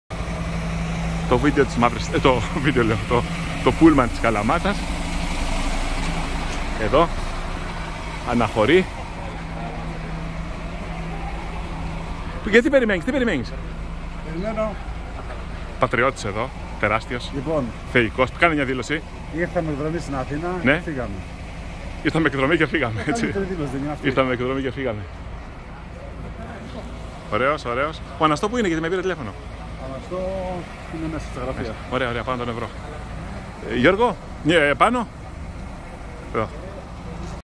"Ήρθαμε... εκδρομή στη Νέα Σμύρνη"!
Το πούλμαν της ΠΑΕ Καλαμάτα, λίγο πριν αναχωρήσει από την Νέα Σμύρνη,  με τον φοβερό οδηγό της Μαύρης Θύελλας, Βασίλη Καμαρινόπουλο να μιλά  στην κάμερα του Sportstonoto!